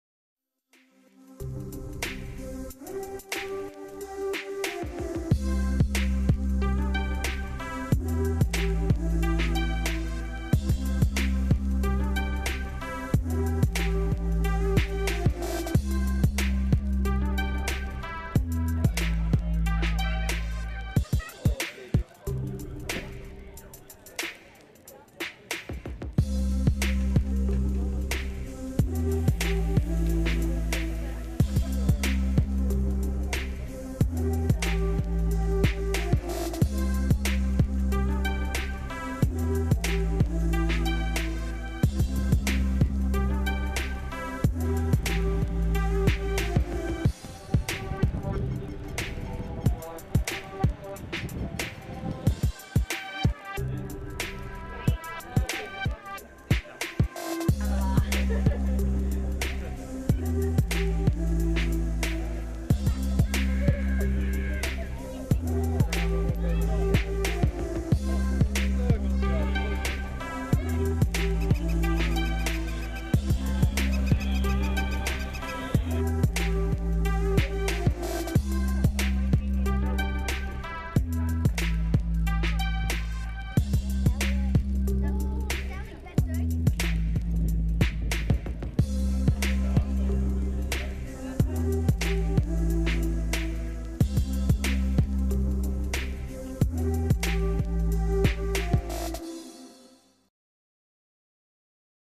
Stalenstraat avondmarkt
Een wandeling over de avondmarkt in de Stalenstraat